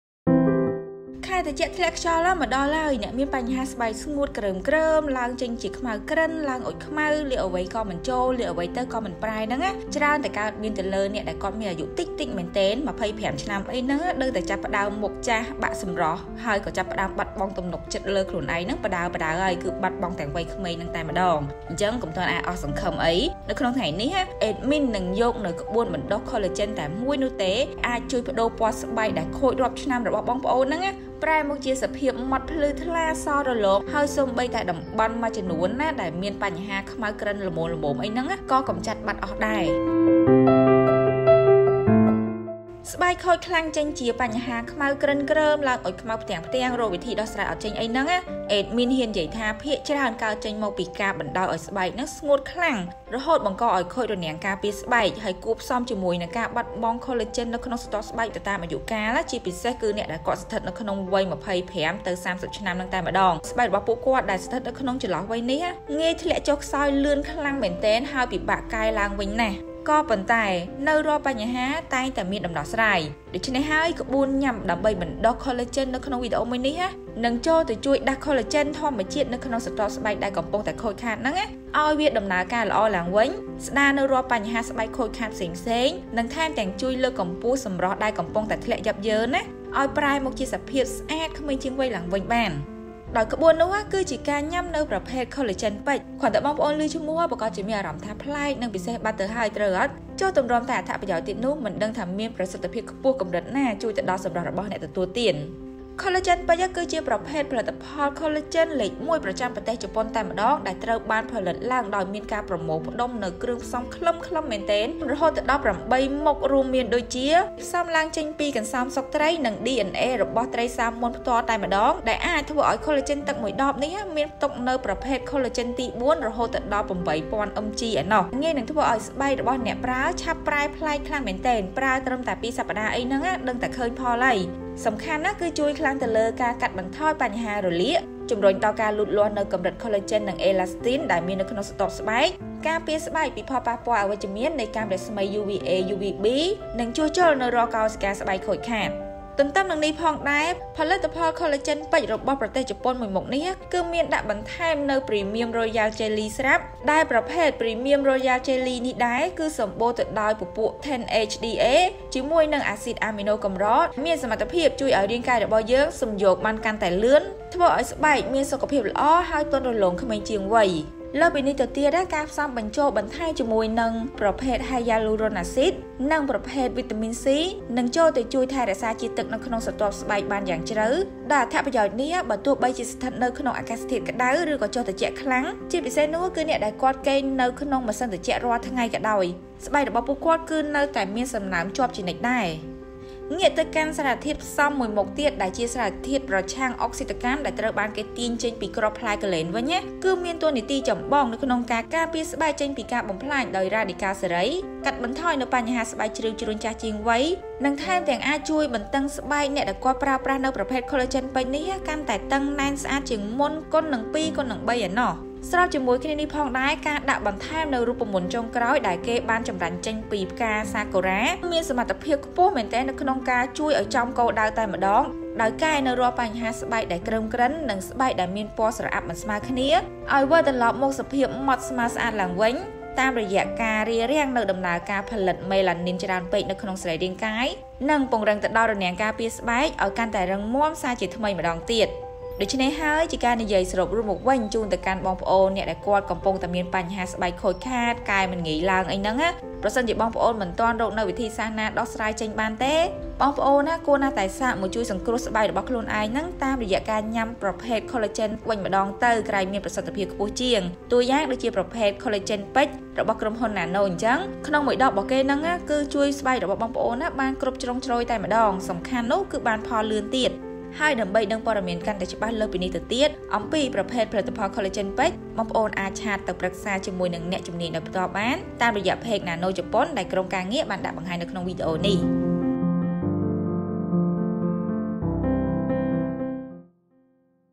ប្រើត្រឹមកន្លះខែនឹងឃើញស្បែកប្រែ! នេះជាក្បួនបណ្តុះខូឡាជែន ជួយកម្ចាត់ចោលបញ្ហាស្បែកស្ងួត ខ្មៅក្រិន
ប្រើត្រឹមកន្លះខែនឹងឃើញស្បែកប្រែ! នេះជាក្បួនបណ្តុះខូឡាជែន ជួយកម្ចាត់ចោលបញ្ហាស្បែកស្ងួត ខ្មៅក្រិន ក្រើម និងឡើងអុជខ្មៅ